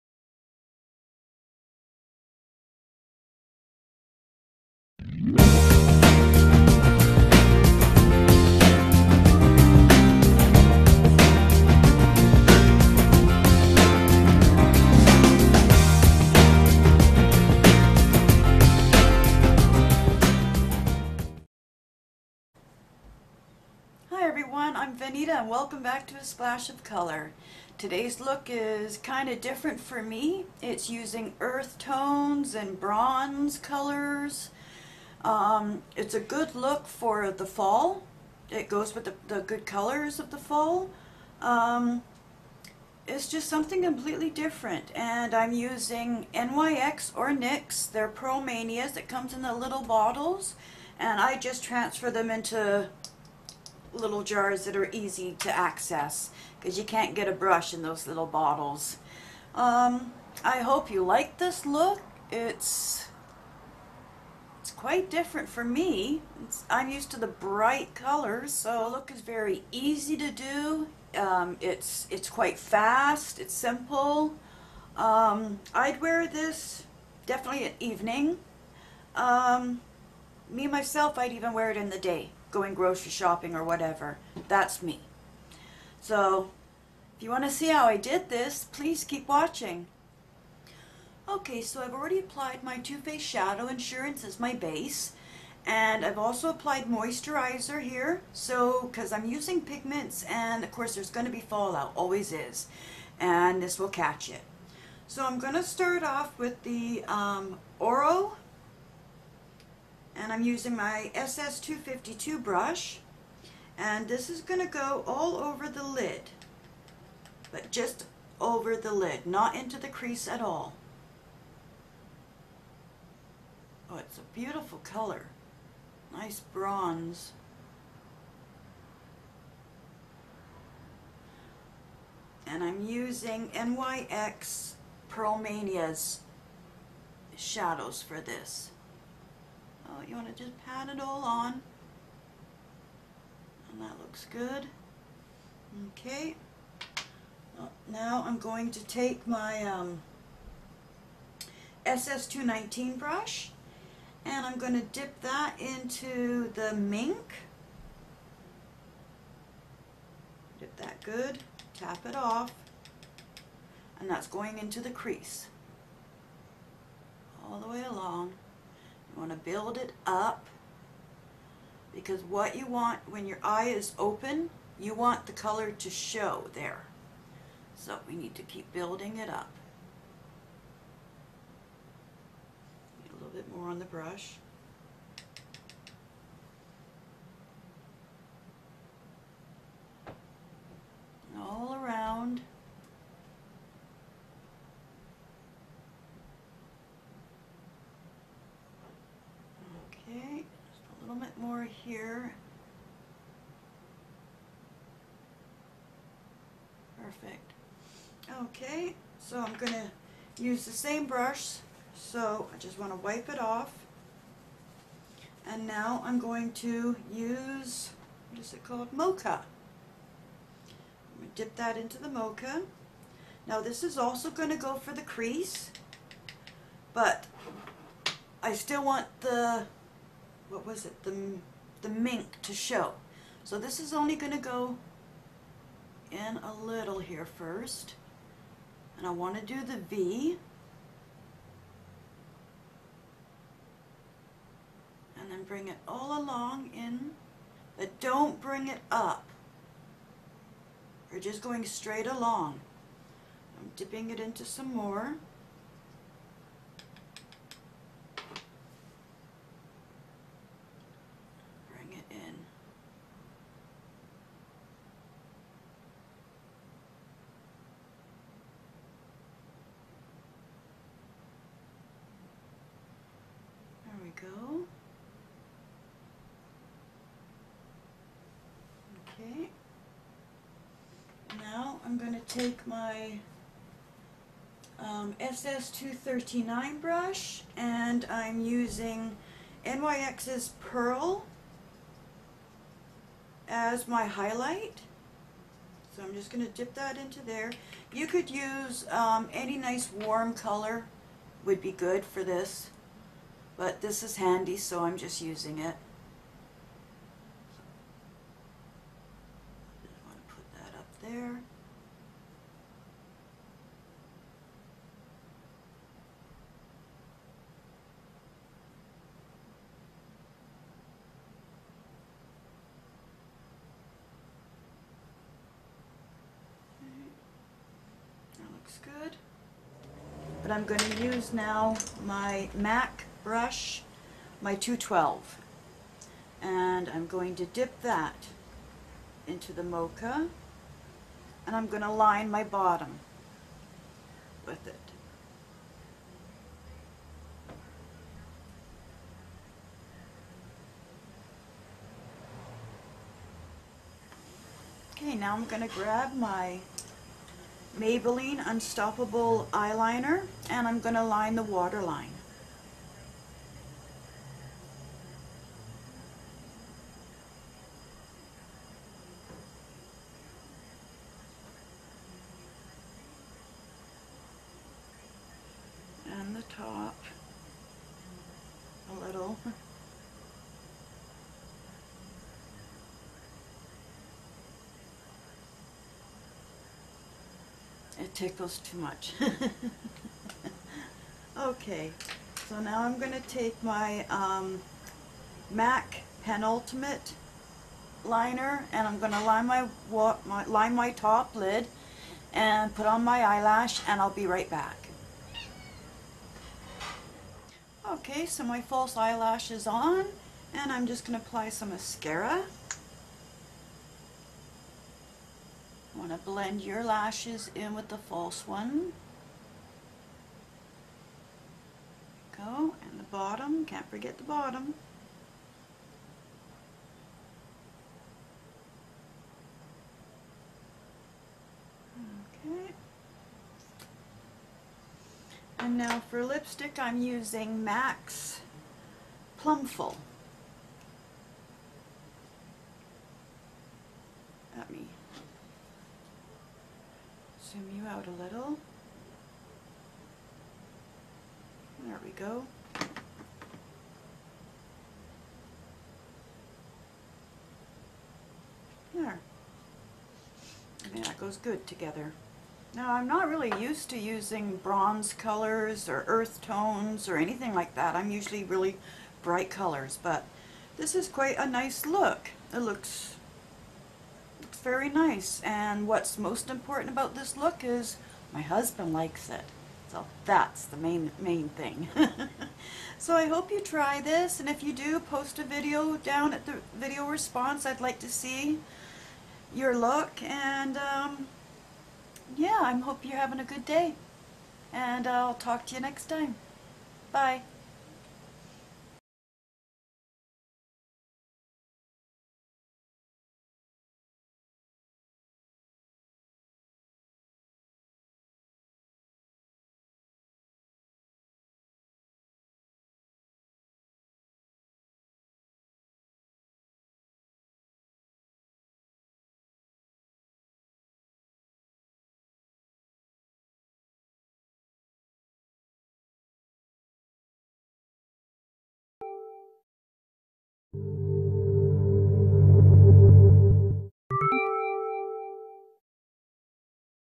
Bronze, Earth Tones
Here is a list of everything i used:
NYX Pearl mania e/s in Oro, Mink, Mocha, Pearl. Maybelline Unstoppable eyeliner in Onyx,
L'Oreal Telescopic Mascara in Black Brown,
MAC Penultimate eyeliner in RapidBlack,
MAC Lipstick in Plumful,
Quo False eyelashes #803